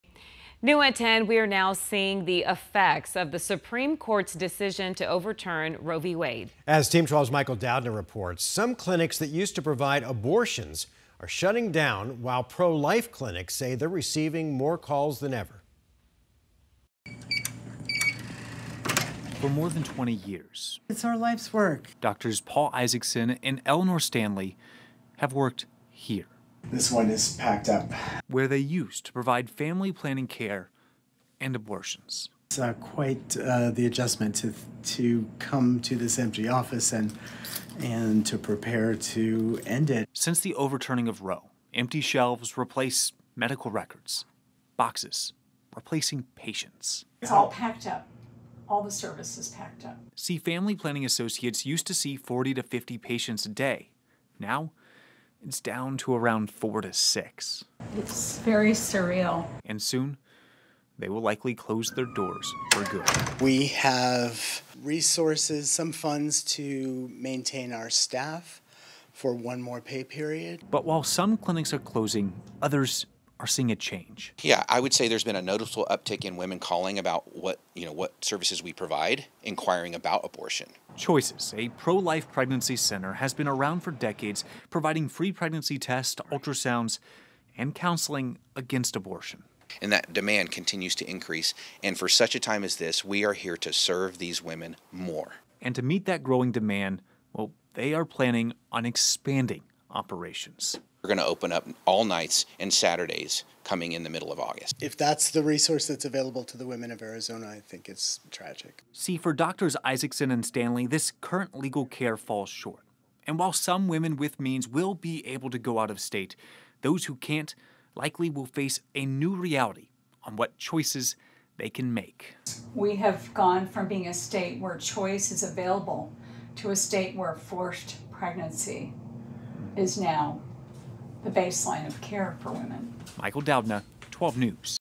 Arizona abortion clinics dealing with the overturning of Roe v. Wade
Family planning clinics are dealing with the overturning of Roe v. Wade. Abortion clinics are struggling while pro-life clinics see uptick in business.